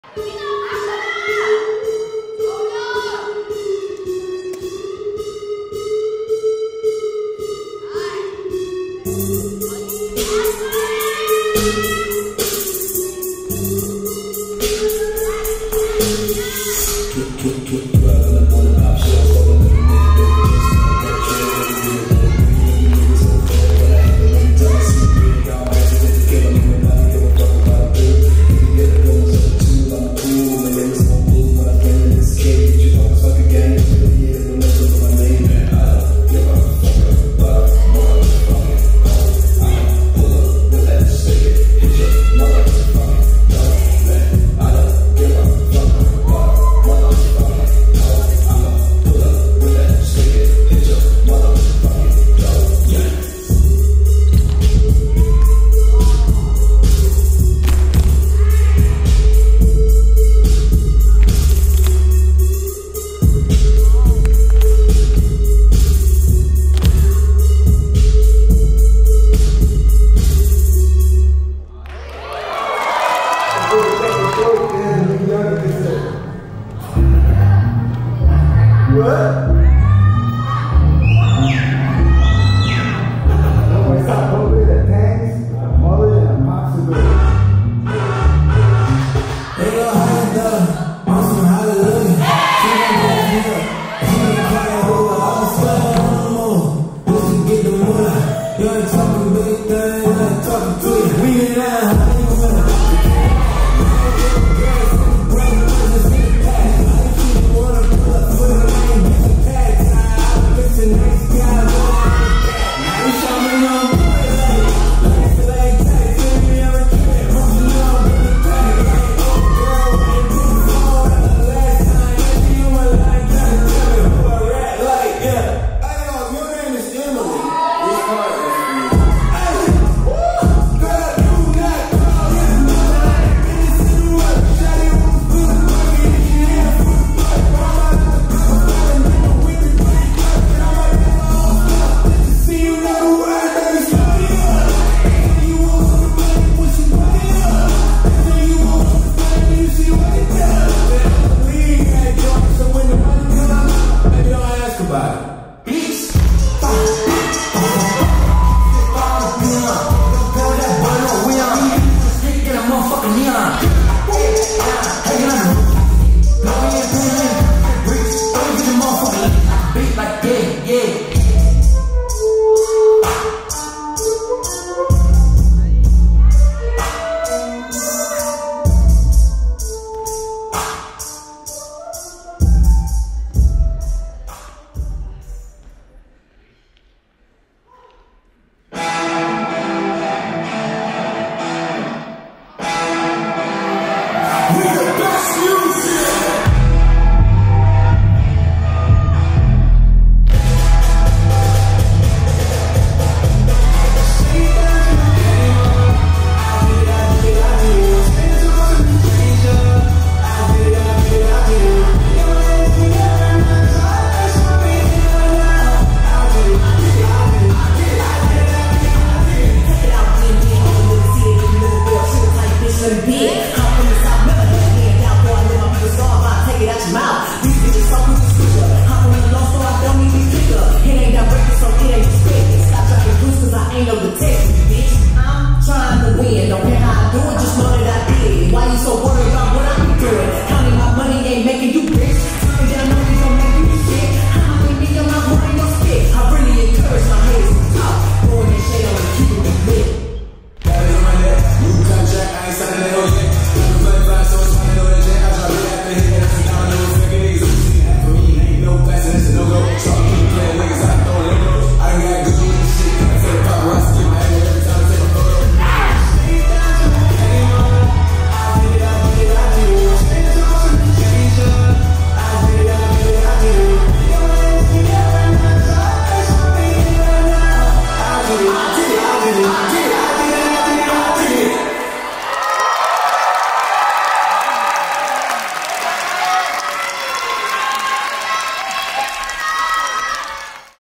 [HARIE] 2023 정기대공연 | 코레오A(뭘 꼬레오?) | Top View
🎡𝙃𝘼𝙍𝙄𝙀𝙑𝙀𝙍𝙎𝘼𝙇 𝙎𝙏𝙐𝘿𝙄𝙊🎢 - 코레오A(뭘 꼬레오?)

안무자) 노소령, 백준형, 한지원
공연진) 나이라, 노소령, 박근욱, 백준형, 에밀리, 윤정휴, 이경미, 이규빈, 이신예,  이유빈, 이유빈, 조조, 차지원, 하다움, 한지원